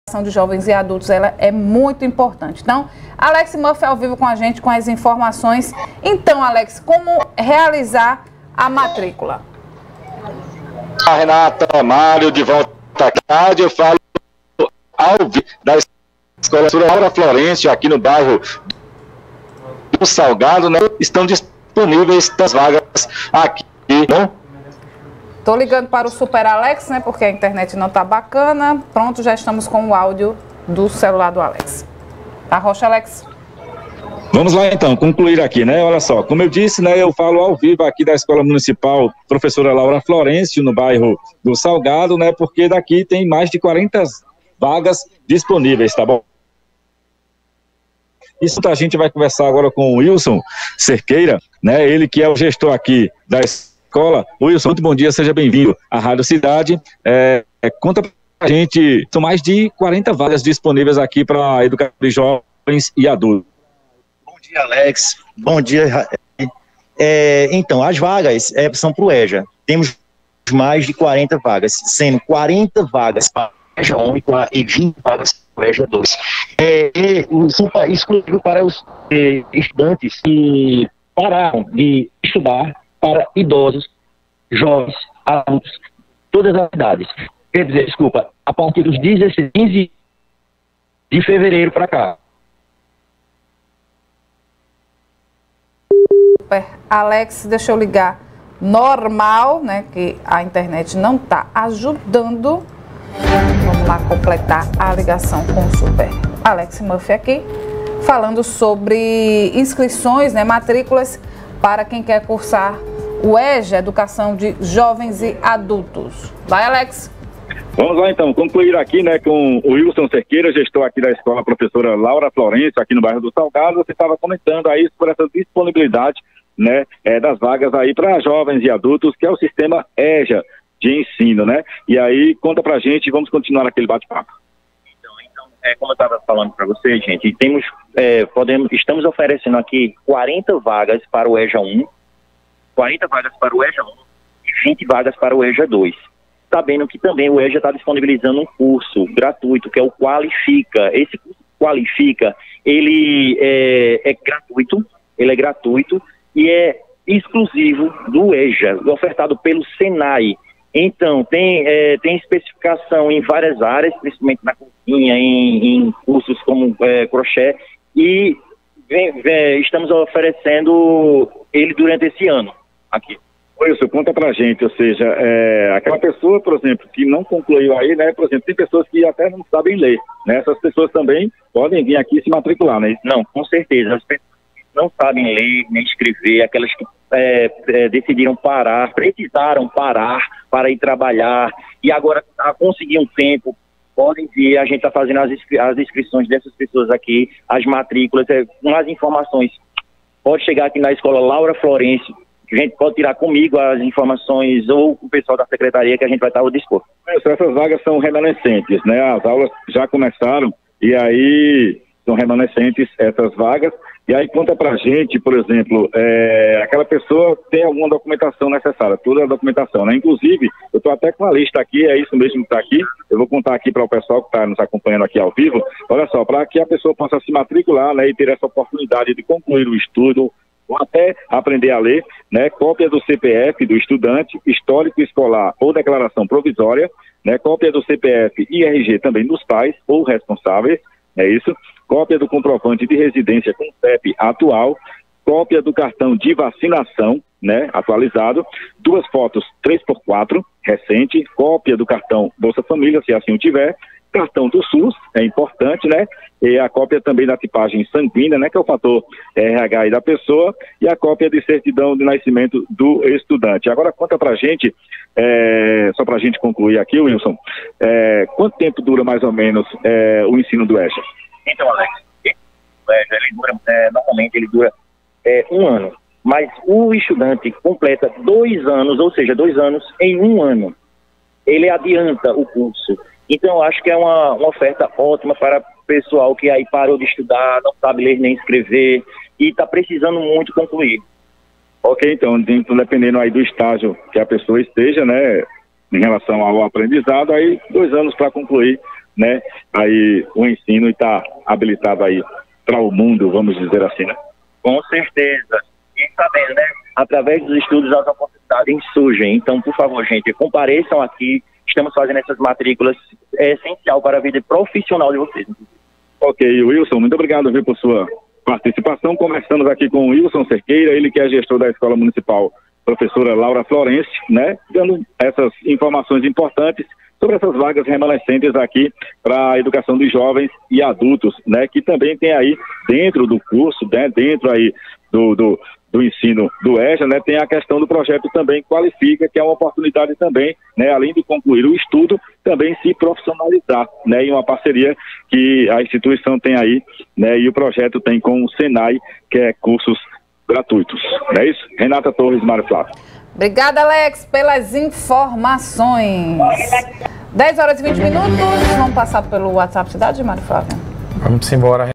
Manhã Cidade - 04.03 - Rede municipal de ensino de Caruaru está com vagas abertas para a EJA
O repórter Alex Murphy explica procedimento para se inscrever.